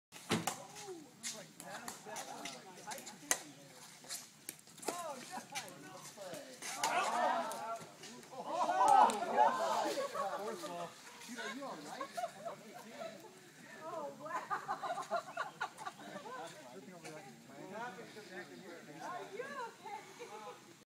Andy meets a wood pile
Andy gets raked over the wood pile thanks to Dave's deep ball